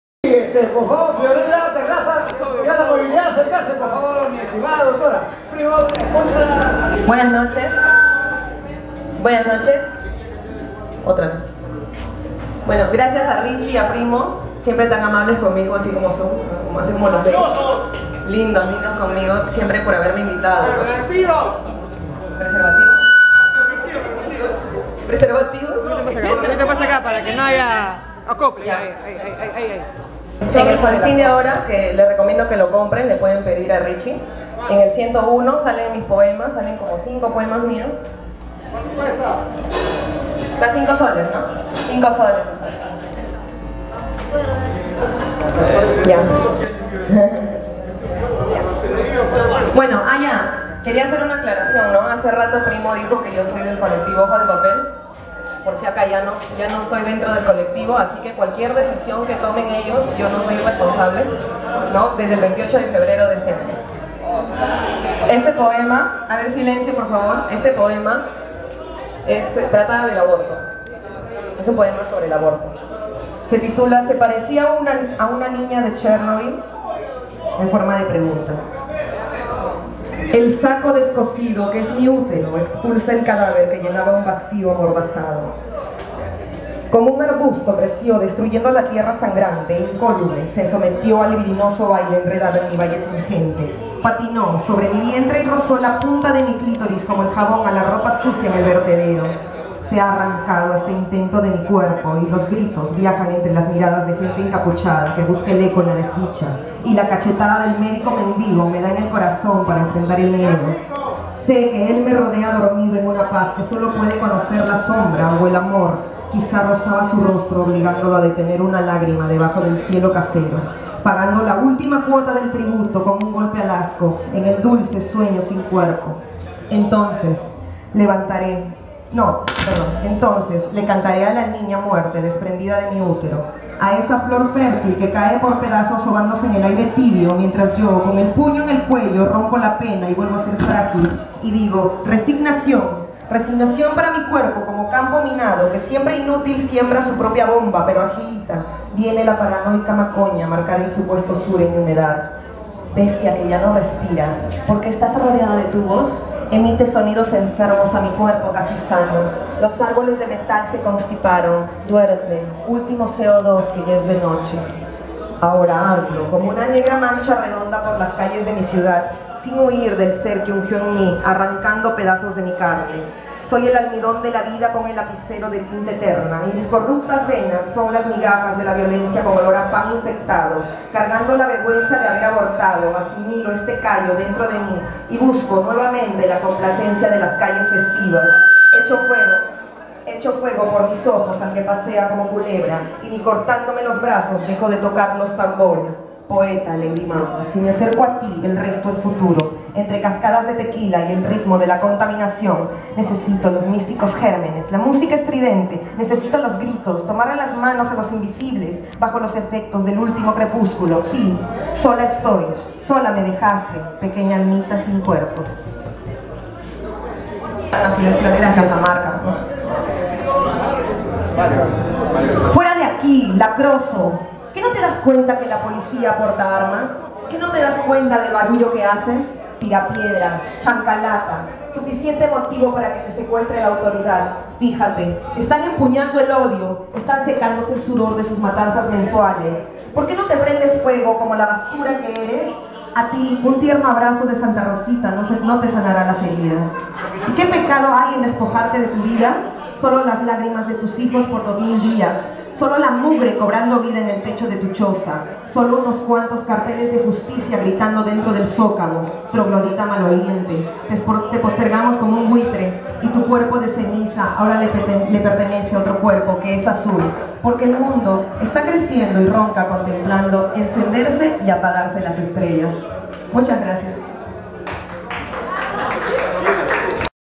"FIORELLA TERRAZAS" CON "LOS POETAS DEL ASFALTO"
"FIORELLA TERRAZAS" CON "LOS POETAS DEL ASFALTO"VIERNES 01 DE MAYO 2015--- LUGAR ..EL CENTRO BAR ,ex años maravillosos --JR CAMANA N-856 -LIMA -ENTRE KILKA Y LA COLMENA ---PRESENTACION DE EL FANZINE DE CONTRAKULTURA POETAS DEL ASFALTO N-101 -EDICION ..LAS SLITS POETIKAS DE LAS SUCIAS KALLES ...TRABAJO KE CONTIENE LIRIKAS DE 10 AMIGAS POETAS .